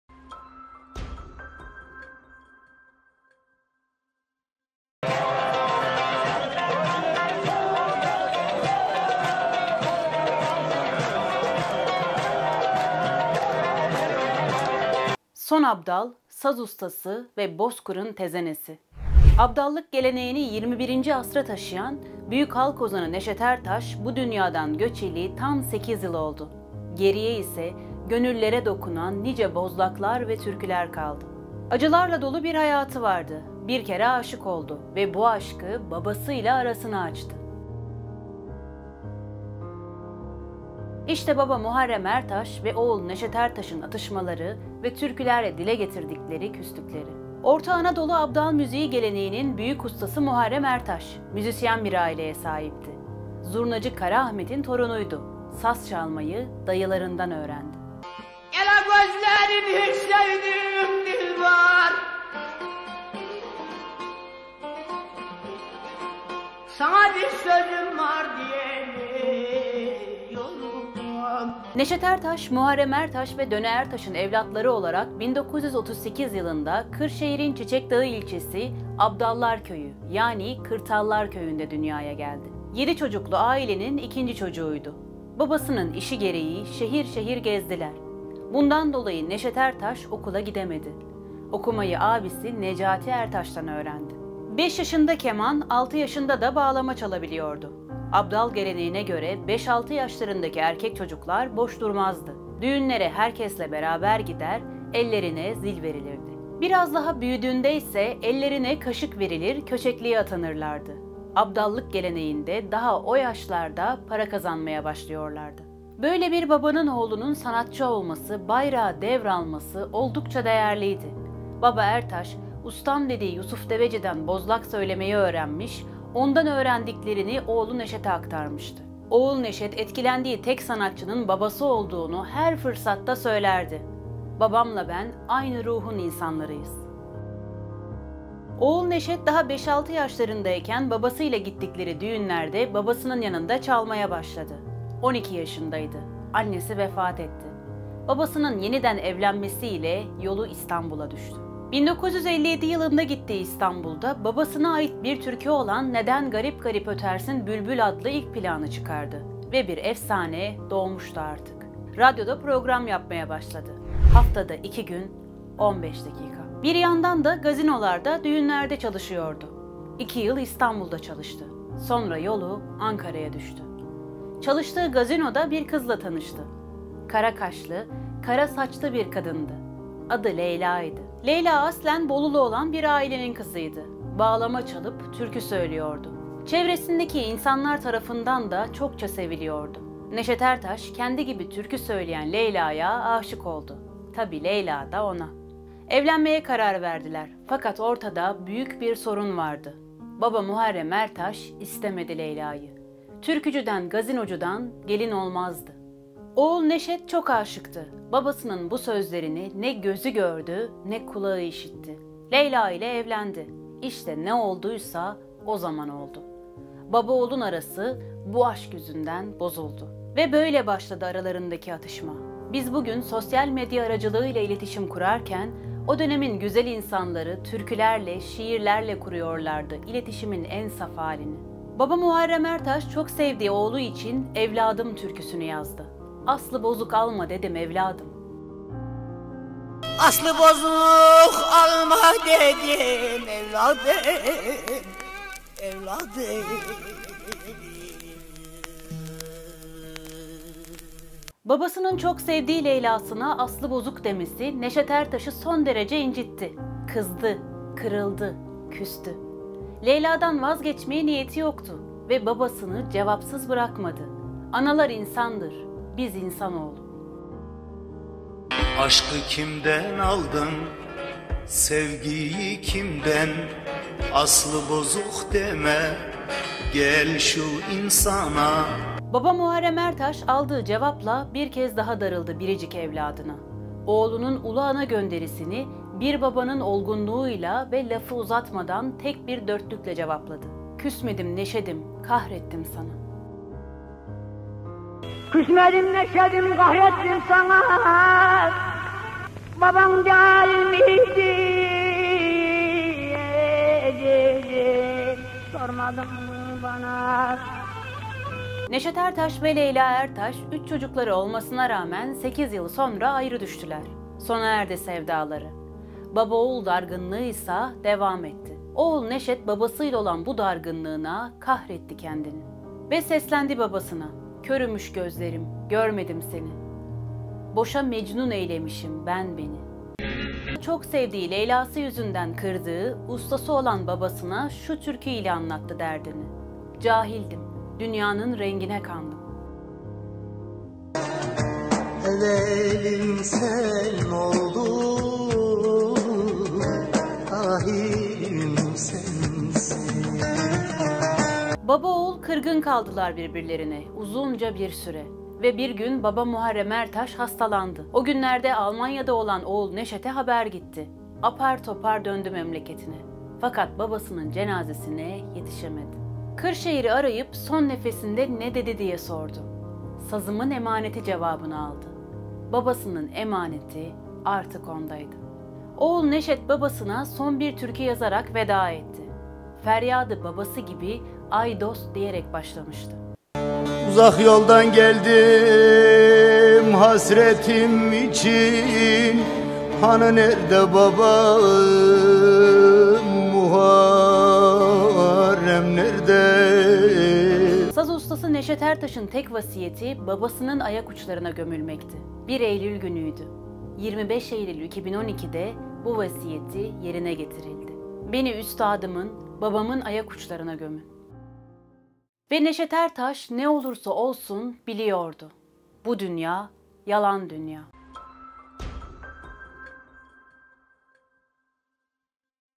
Bir Baba-Oğul Kavgası... Türkülerle Dile Gelen Küslük
Abdallık geleneğini 21. asra taşıyan, büyük halk ozanı Neşet Ertaş'ın acılarla dolu bir hayatı vardı.
Bir kere aşık oldu ve bu aşkı babasıyla arasını açtı…
İşte baba Muharrem Ertaş ve oğul Neşet Ertaş’ın atışmaları ve türkülerle dile getirdikleri küslükleri…